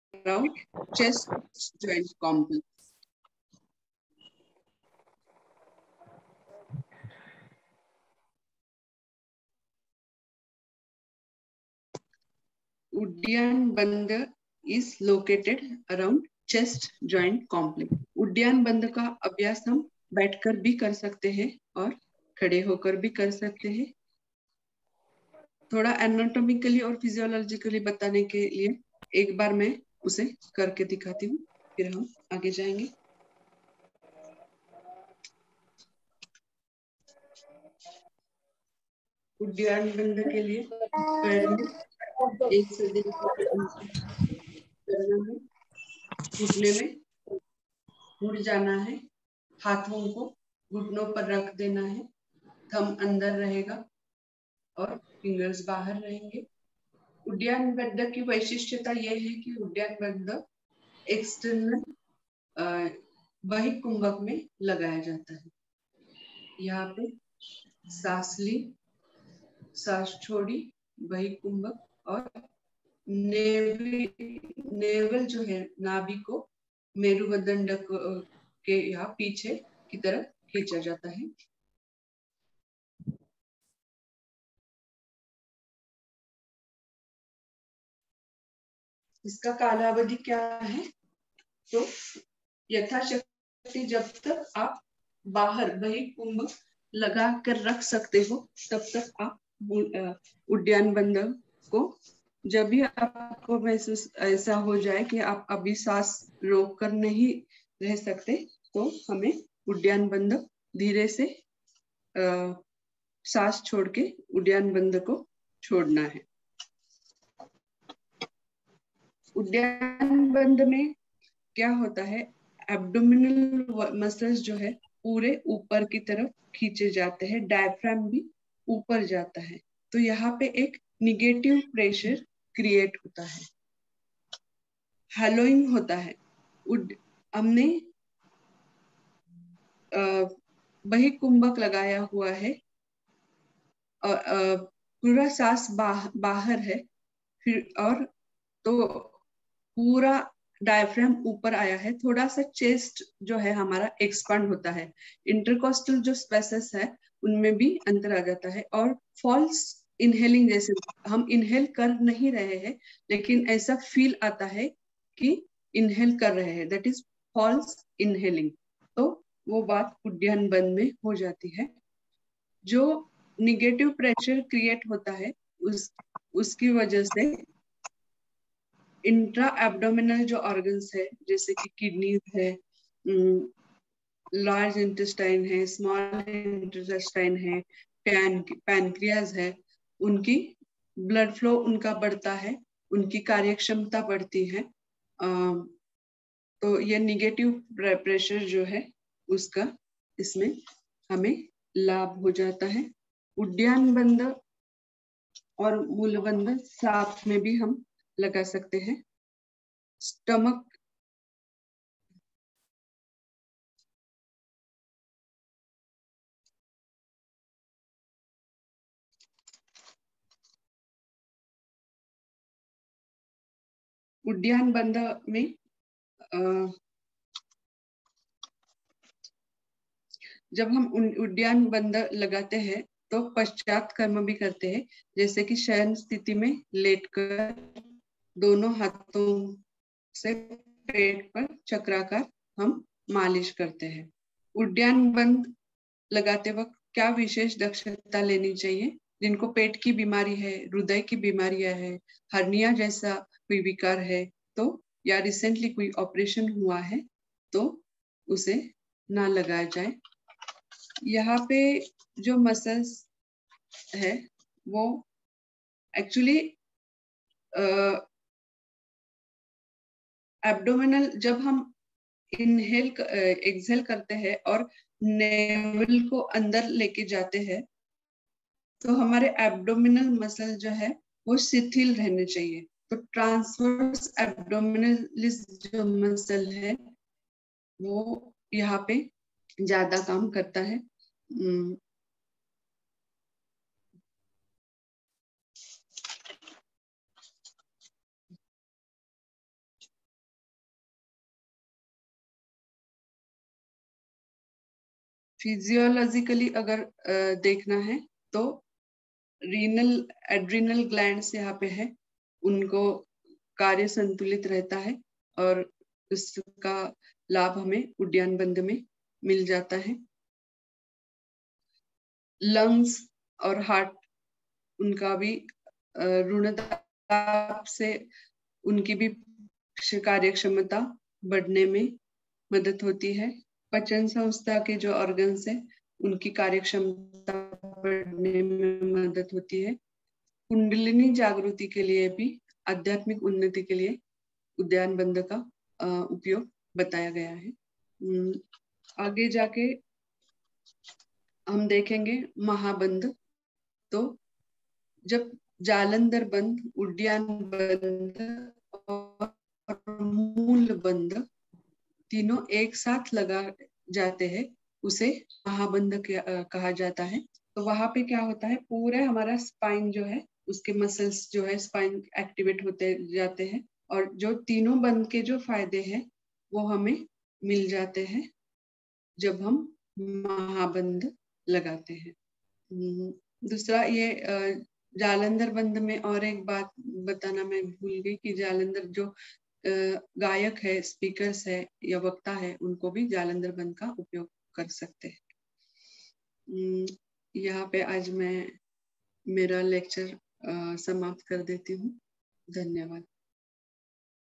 Yoga Lecture - IDY 22 - Uddiyana & Maha Bandha - Dt. - 20.06.22
YCB Exam Guidelines Lecture Series - Speaker Dr. Vrunda Kulkarani.